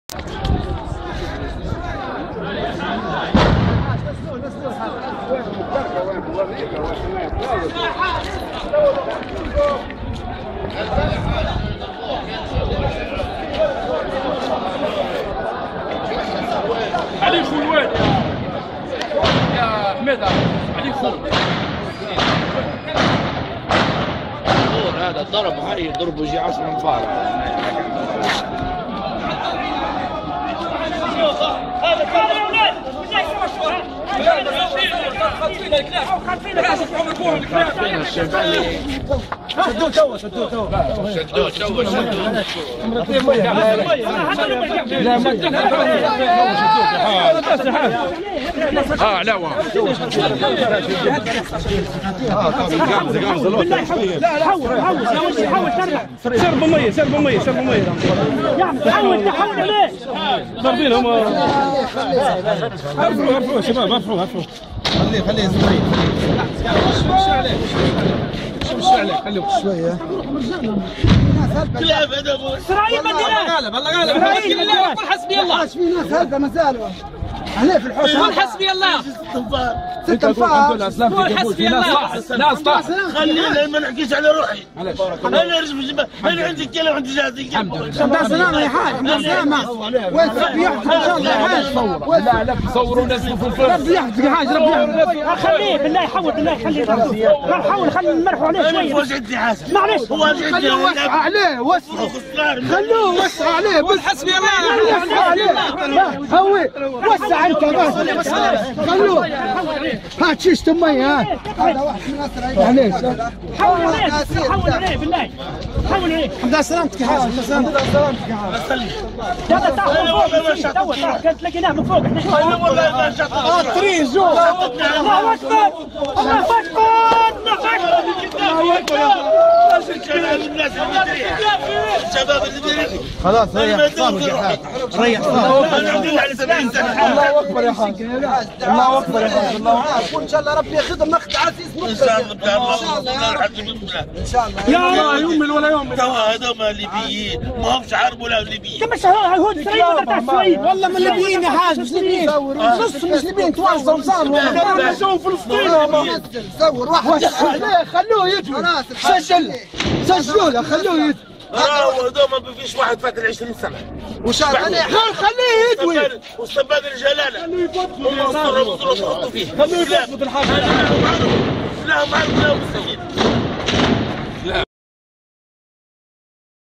‫الخـــالة ‬
فيديو جديد و حصري يعرض لاول مرة يوضح إختطاف رجل كبير فن السن من قبل مِليشيات غرغور اثناء المظاهرة و تمكنه من الفرار منهم و يقول بأن هناك حولي ستة أشخاص مازالوا محتجزين و يحكي عن تعرضه لسب و الشتم و الاهانة